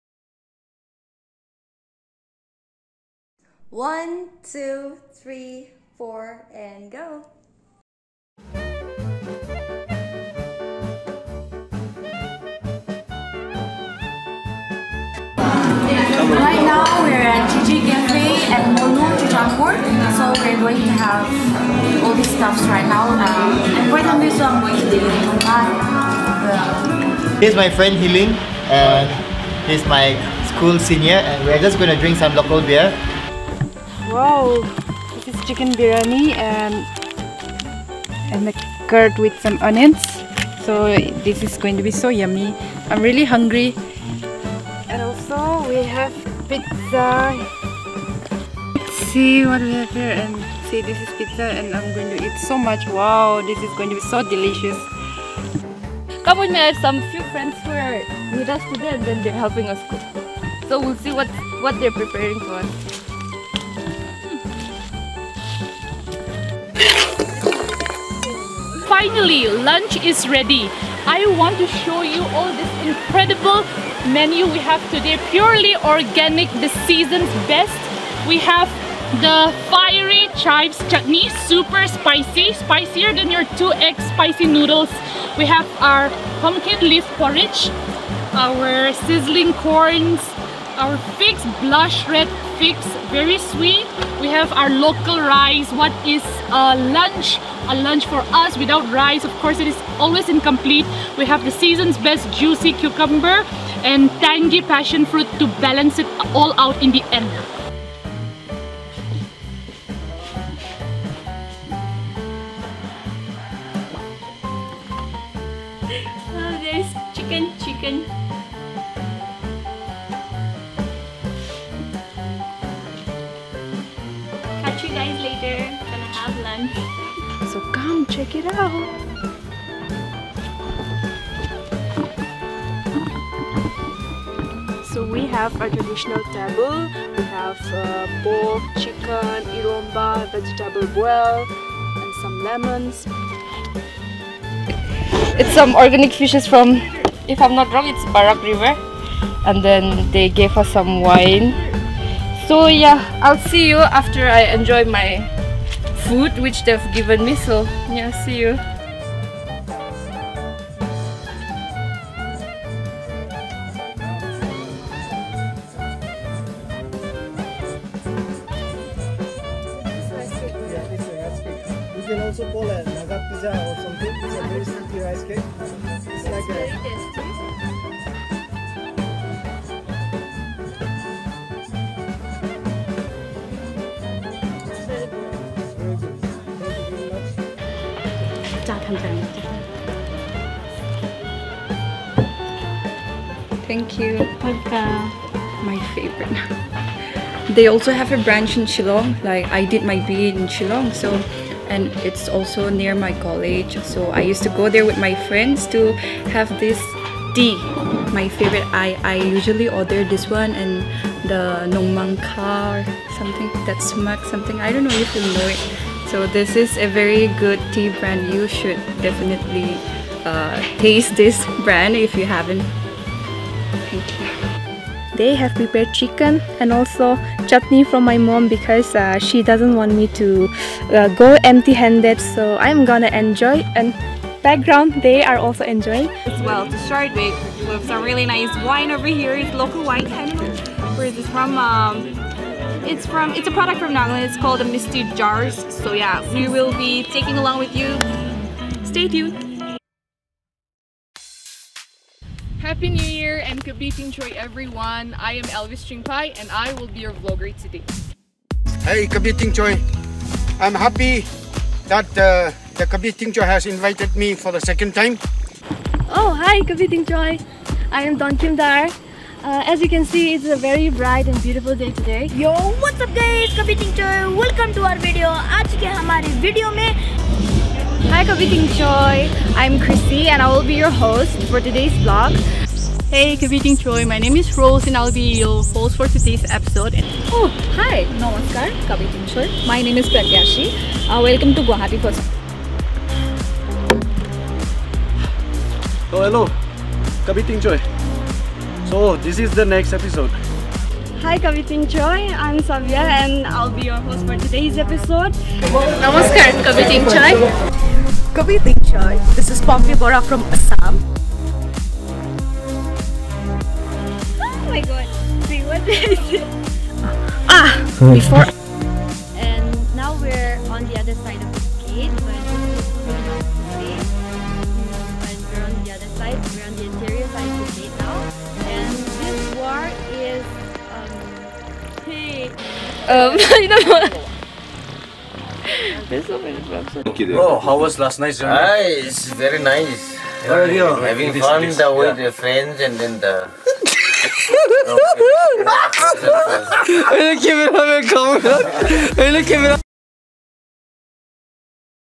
MEMORIES FROM 2021 ll A LOOK BACK ll KABI TINGCHOI
As we look forward to 2022, here are some memories from our journey in 2021.